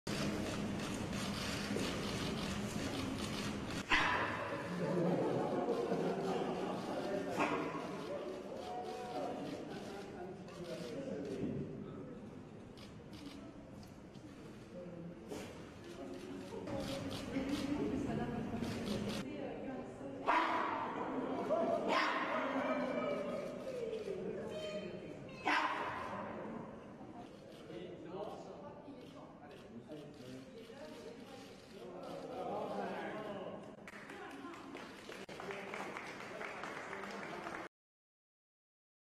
Panda cub growls and jumps at France’s first lady, Brigitte Macron
France's first baby panda has been named Yuan Meng ('accomplishment of a dream') at a ceremony attended by Chinese dignitaries and the French first lady, Brigitte Macron.  The ceremony at Beauval zoo, south of Paris, was considered a diplomatic event,  but the four-month-old male cub decided otherwise, making a growling sound and jumping towards Macron when she put her hand over a glass wall to pet the animal.
The baby panda's mother, Huan Huan, was artificially inseminated from partner Yuan Zi this spring. Both are at the zoo on a 10-year loan from China, and their offspring officially belong to the Chinese government. The cub will be sent to a Chinese panda reserve when it is weaned